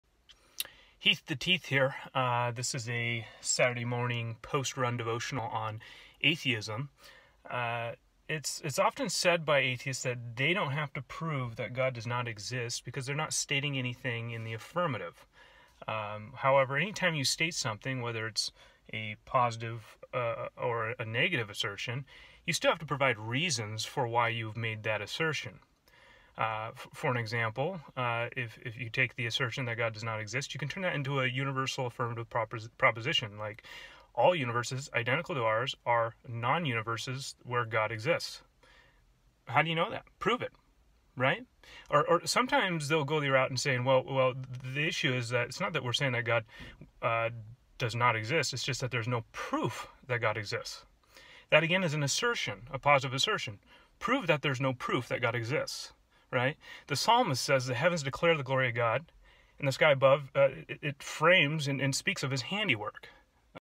On Atheism, a Saturday Morning Post Run Devotional.
Heath breaks down common atheistic approaches on the existence of God.